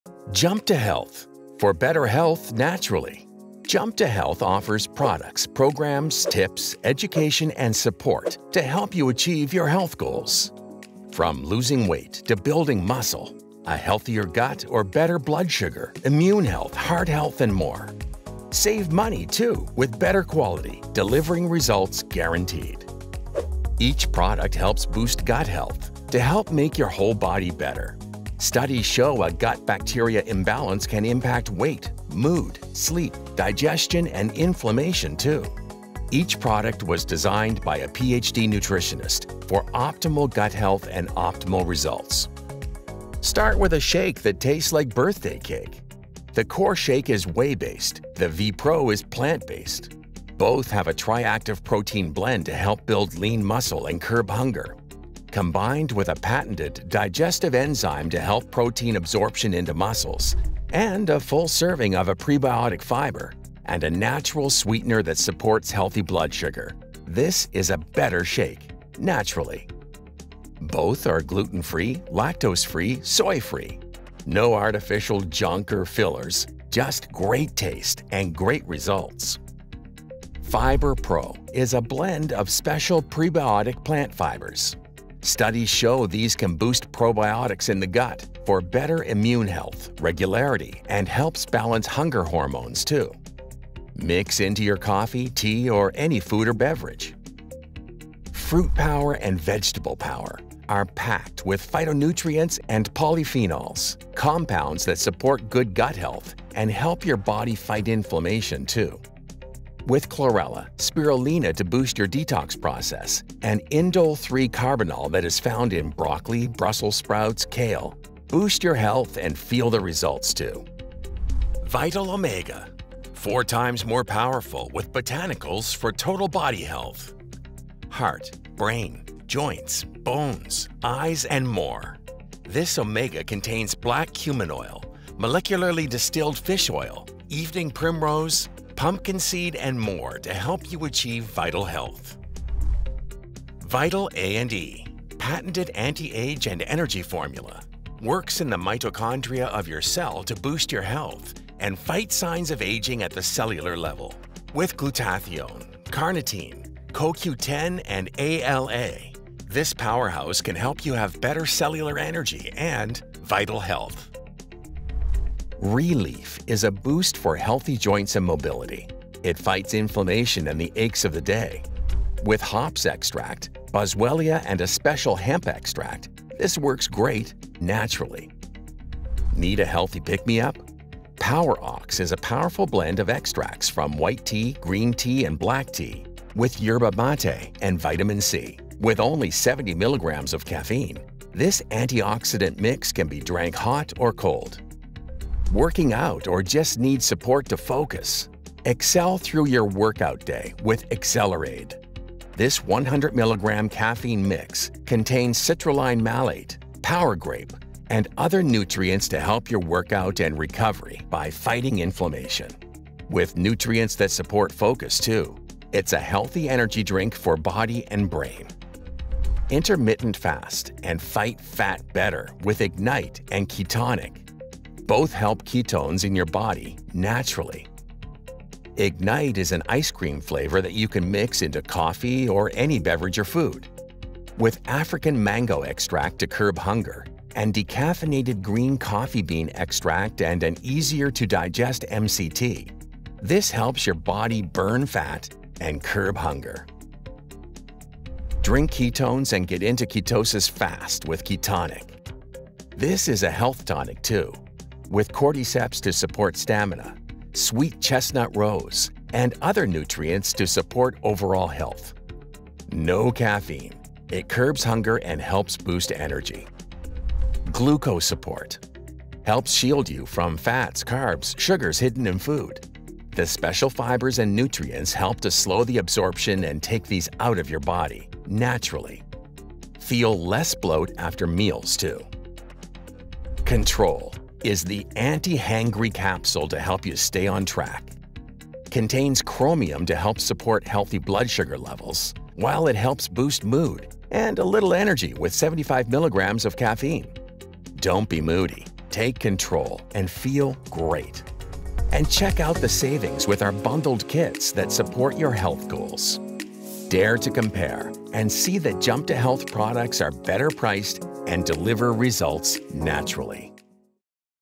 The Jump To Health™ Products!
A concise overview al all our Jump To Health™ products! Our products are custom formulated to support your health journey and deliver results! This 5 minutes can dramatically impact your health!